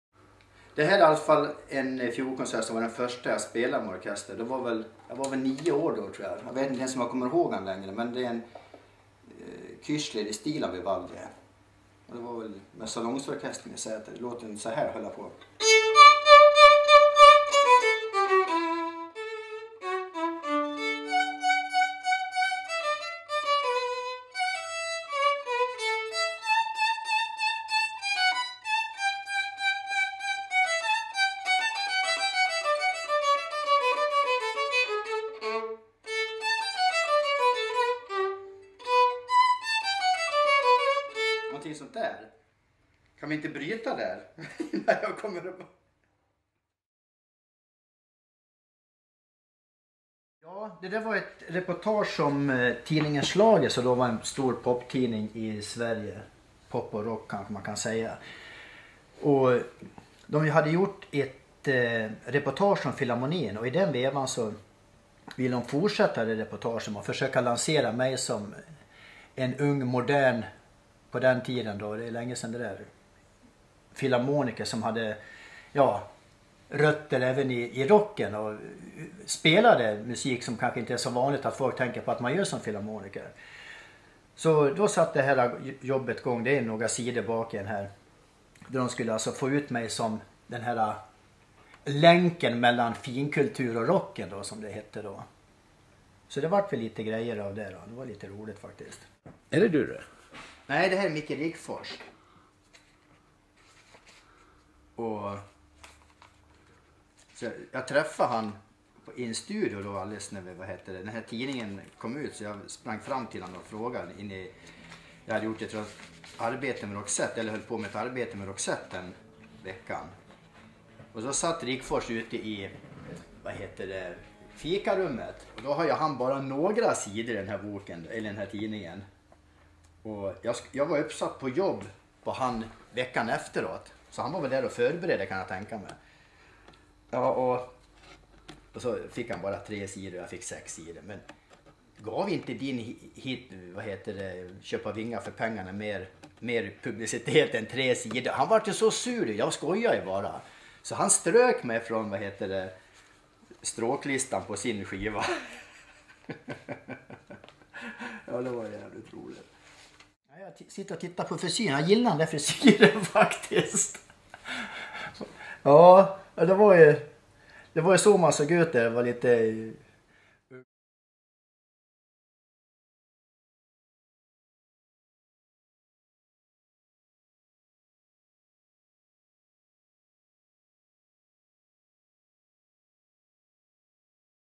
Thomas Lundblad
Mycket tidigt kom musiken in i Thomas liv. Han var bara några år när han gav sin första konsert på skohorn.

Hans far var rektor på musikskolan och hans mamma pianolärare.

I alla fall blev fiolen huvudinstrument. Han kom att spela i filharmonikerna som Sveriges Radios symfoniorkester.

Han var även hockeytränare samtidigt. En dag blev det för mycket för honom och han gick in i "väggen". Thomas har nu sakta kommit tillbaka in i musiklivet igen.